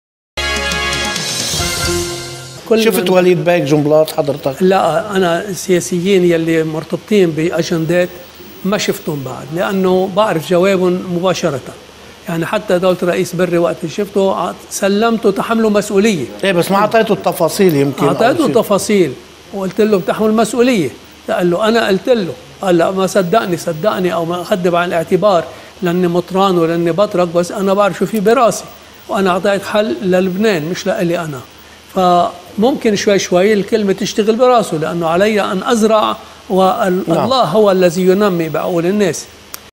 15- علي ان أزرع وعلى الله أن ينمي ما يقع في عقول الناس.mp4
استنادا إلى رؤية الجمهورية الخامسة للأب ميخائيل روحانا الأنطوني، وقبل يوم من الجلسة الثانية عشرة لانتخاب رئيس للجمهورية اللبنانية والمعروف سلفا أنها لن تؤدي إلى انتخاب رئيس، طرح بعض النواب والمحللين السياسيين موضوع "الخِيار الثالث" المفروض وجوده بين المتخاصمَين خدمة للديمقراطية. يأتي جواب الأب الدكتور روحانا بوجوب طرح فكرة تعديل الدستور أولا. فالحل الدائم والسليم والمتكامل ليس في شخص الرئيس الذي لا سلطة لديه، بل في إعادة النظر بالدستور القائم على اتفاق الطائف المشؤوم.